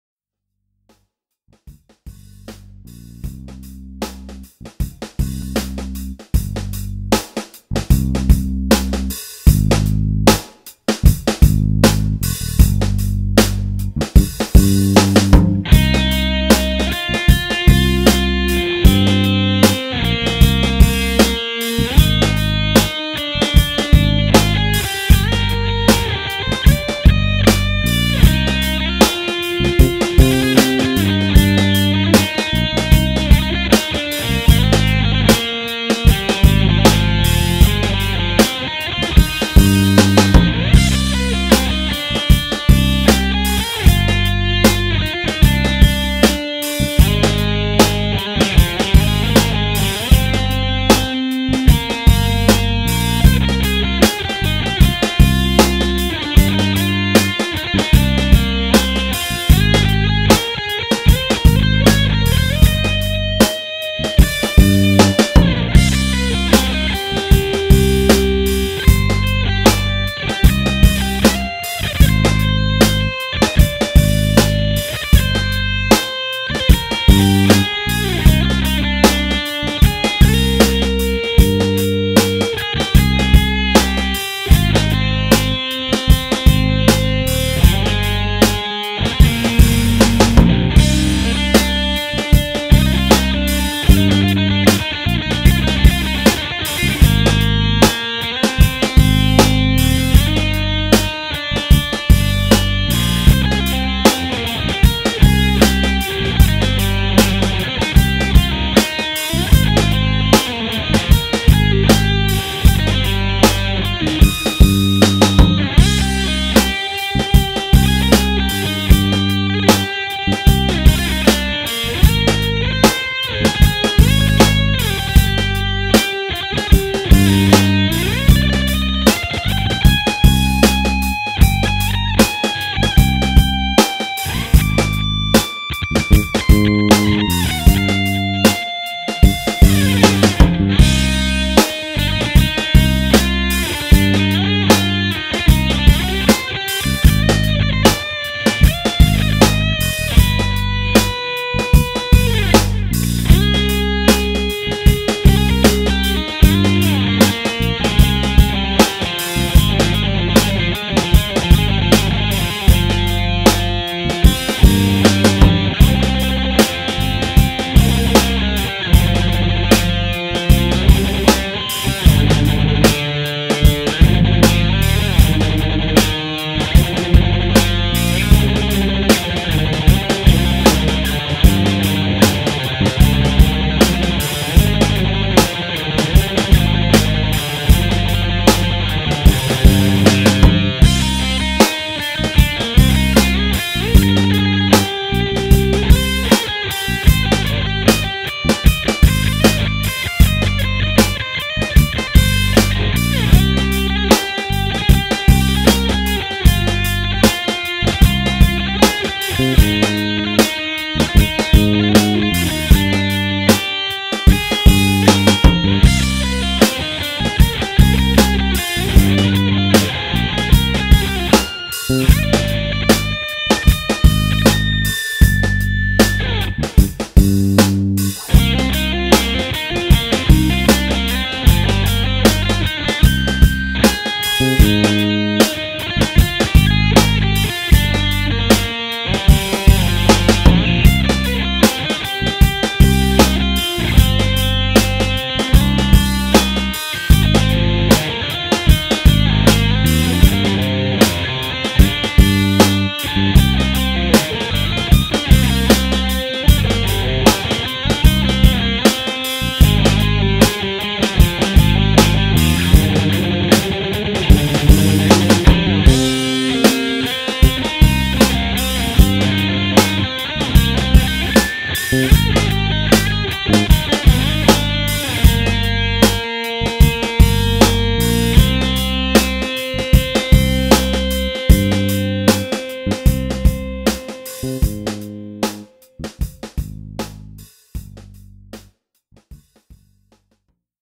DigiTech Trio Pedal Demo
got a new guitar pedal the Trio DigiTech so i decided to do a little demo it's basically a band in a pedal and i love it so far be sure to like share and subscribe